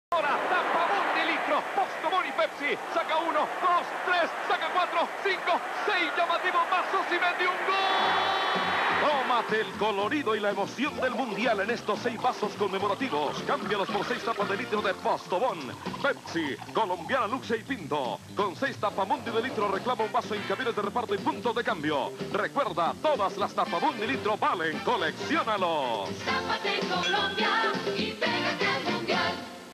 Destapate Postobon
Comercial noventero de Postobón y sus vasos conmemorativos de Italia 1990